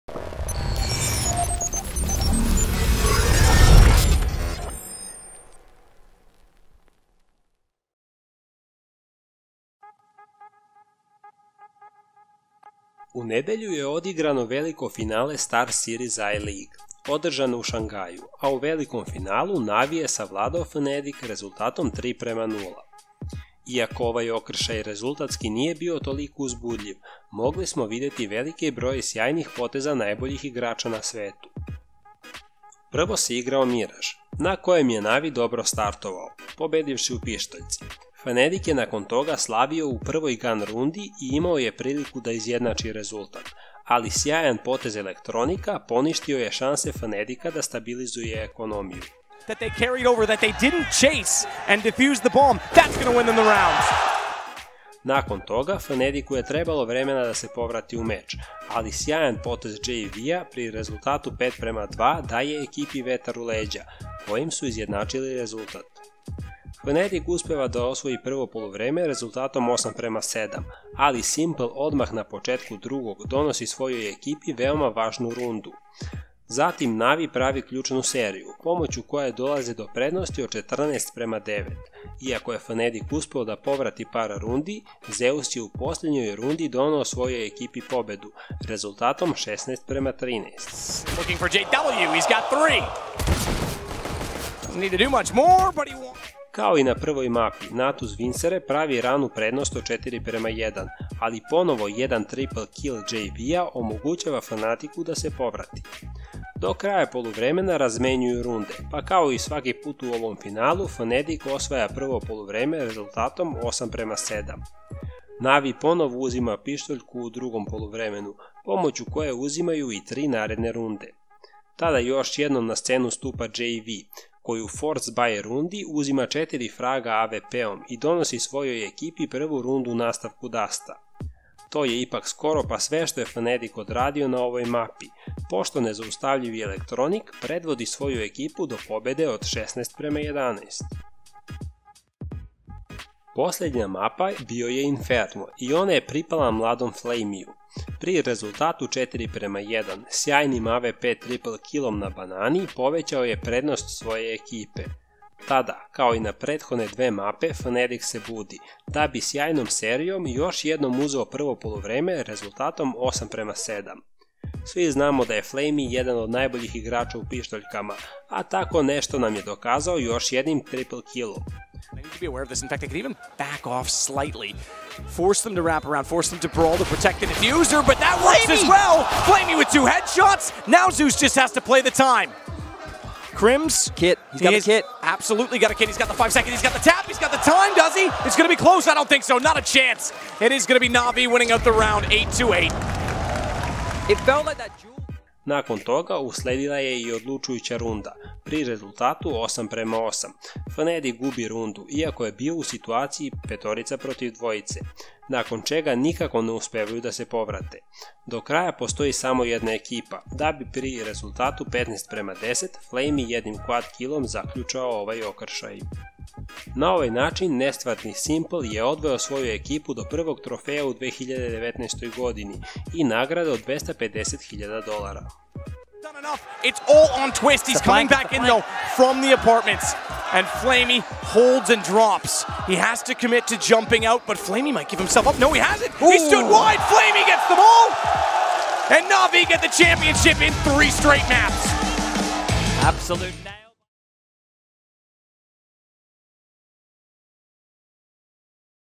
Na’Vi počistio fnatic u finalu StarSeries i-lige - RUR analiza
Nadali smo se jednom veoma zabavnom finalu, između dva giganta CS:GO scene, s obzirom da smo posle duže vremena gledali finale u BO5 formatu, ali iako fnatic nije bio toliko loš, S1mple i electronic su bili nezaustavljivi u velikom finalu StarSeries i-lige.